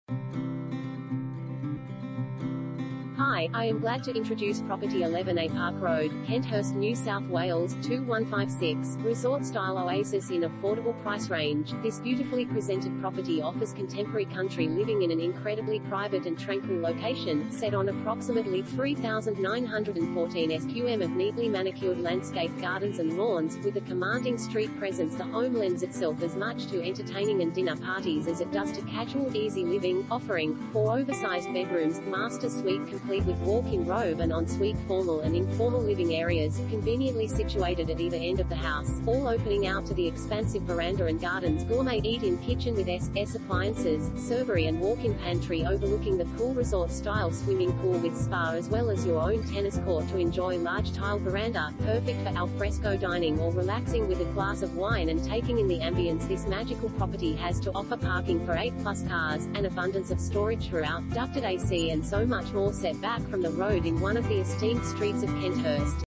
11a Park Road, Kenthurst NSW 2156 | AuPropertyReport.Com
11a Park Road, Kenthurst NSW 2156
Property Type: house
Bedrooms: 4
Bathrooms: 4
Car Space: 8
Resort style oasis in affordable price range